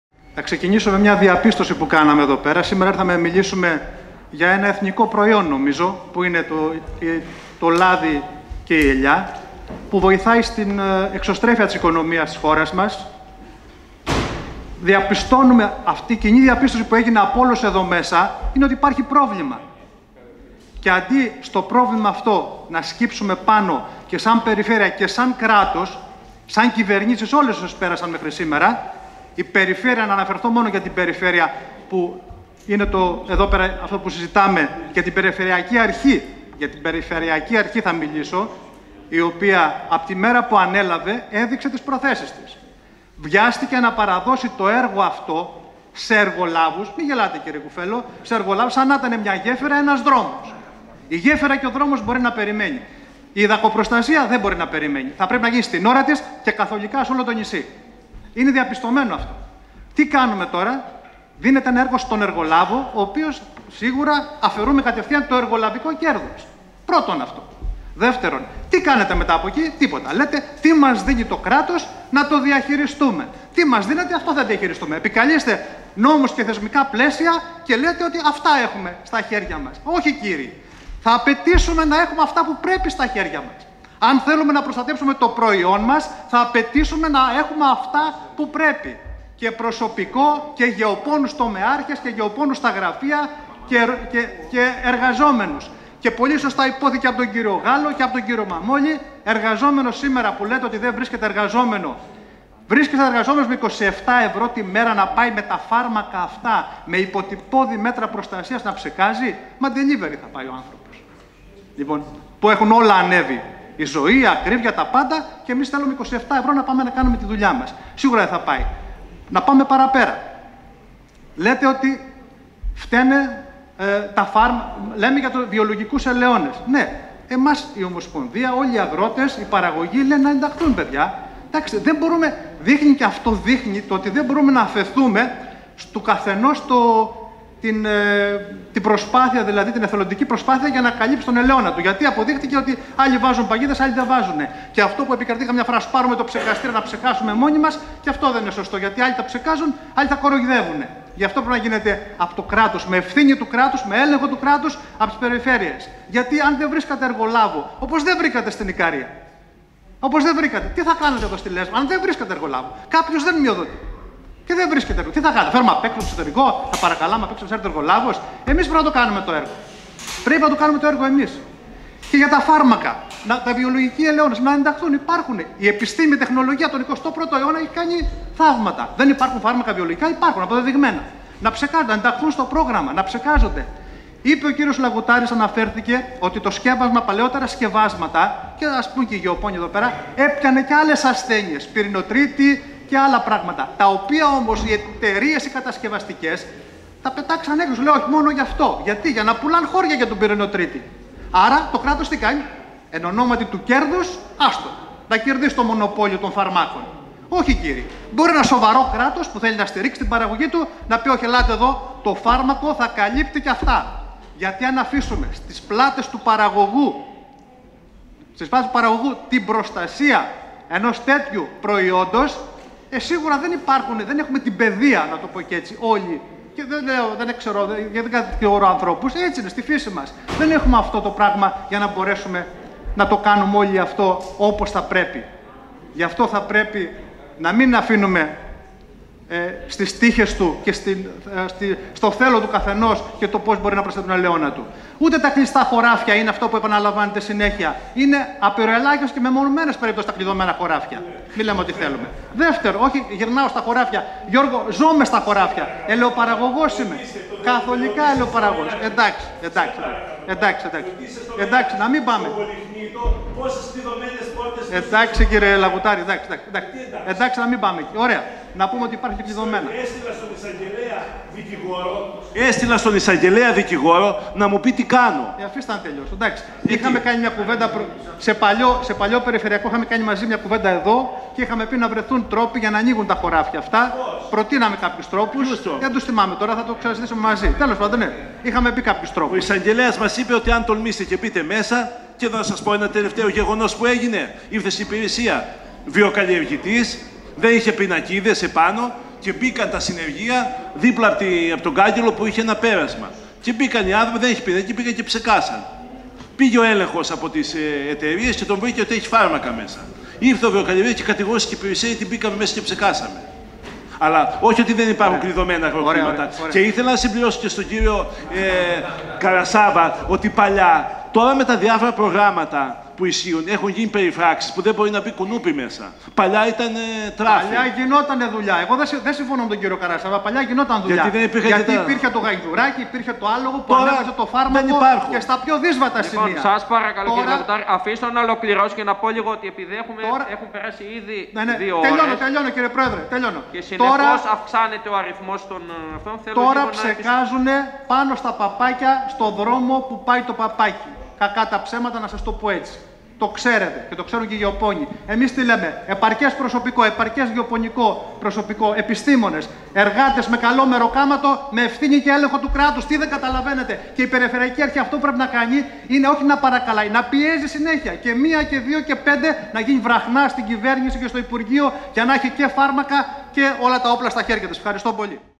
Η τοποθέτηση του Γραμματέα Αγροτικών Συλλόγων Λέσβου Στρατή Κόμβου
Άκαρπη η συζήτηση για την δακοκτονία